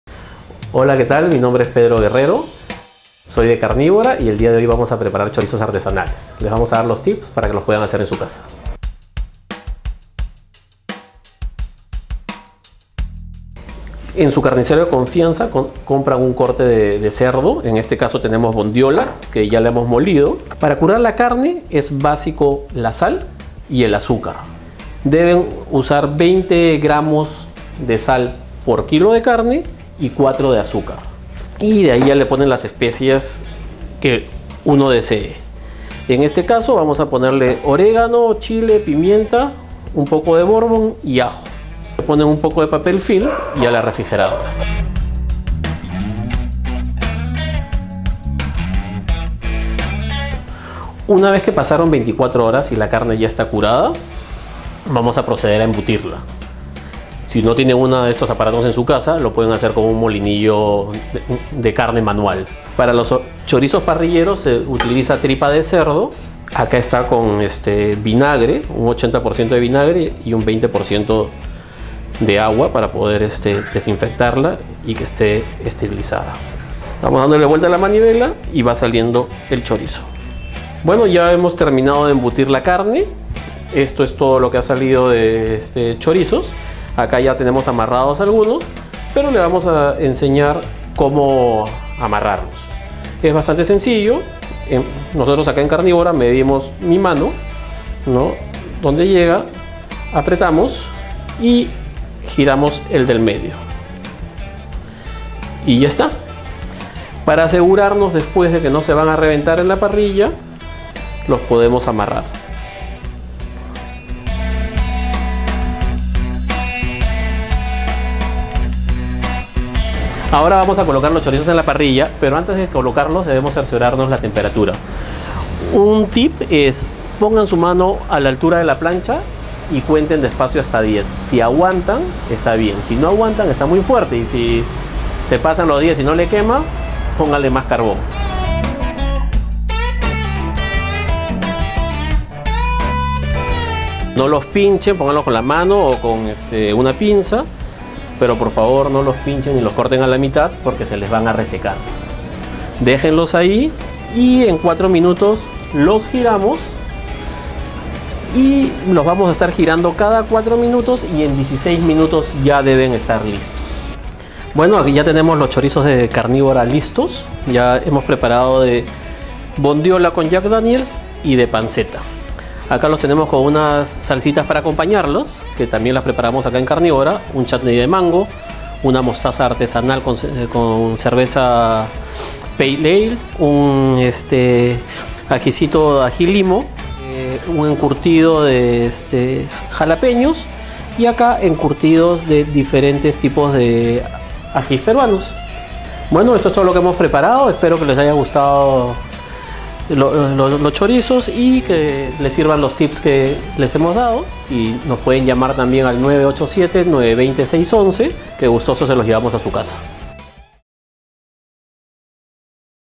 Aprenda a elaborar un chorizo y a cocinarlo
Pedro Guerrero, fundador de Carnívora, comparte sus consejos para elaborar chorizo casero y prepararlo como un maestro